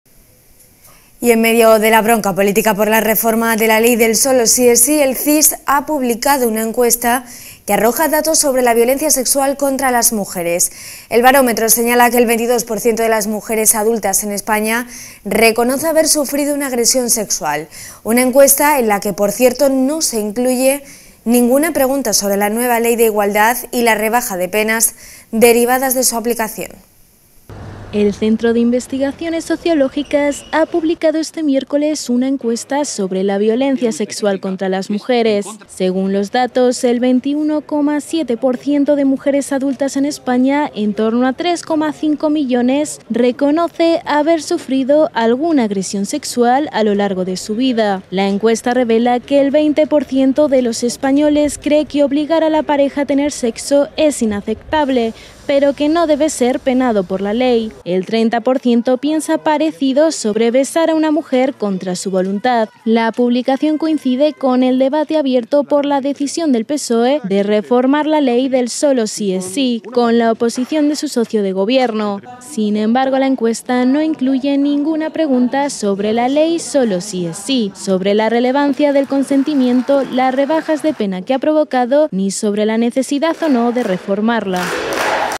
🔴Noticia - Un 21,7% de mujeres españolas reconoce haber sufrido una agresión sexual
El Centro de Investigaciones Sociológicas ha publicado este miércoles una encuesta sobre la violencia sexual contra las mujeres. Según los datos, el 21,7% por ciento de mujeres adultas en España, en torno a 3,5 millones, reconoce haber sufrido alguna agresión sexual a lo largo de su vida. Sin embargo, la encuesta no incluye ninguna pregunta sobre la Ley de Libertad Sexual o del 'Sólo Sí es Sí', sobre la relevancia del consentimiento, las rebajas de penas que ha provocado, ni sobre la necesidad o no de reformarla.

English:

The Center for Sociological Research has published this Wednesday a survey on sexual violence against women. According to the data, 21.7% percent of adult women in Spain, around 3.5 million, acknowledge having suffered some sexual assault throughout their lives. However, the survey does not include any questions about the Law on Sexual Freedom or 'Only Yes is Yes', about the relevance of consent, the reductions in sentences that it has caused, or about the need or not to reform it.

Live streaming